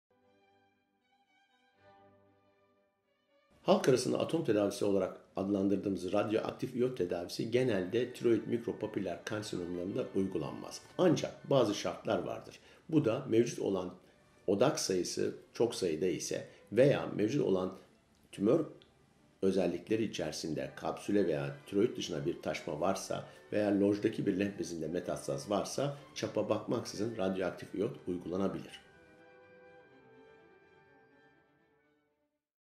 Mikropapiller Tiroid Kanserleri Ameliyatı Sonrası Atom Tedavisi Gerekir Mi ?
Prof. Dr. Serdar Tezelman "Mikropapiller Tiroid Kanserleri Ameliyatı Sonrası Atom Tedavisi Gerekir Mi ?" sorusunun yanıtını veriyıor.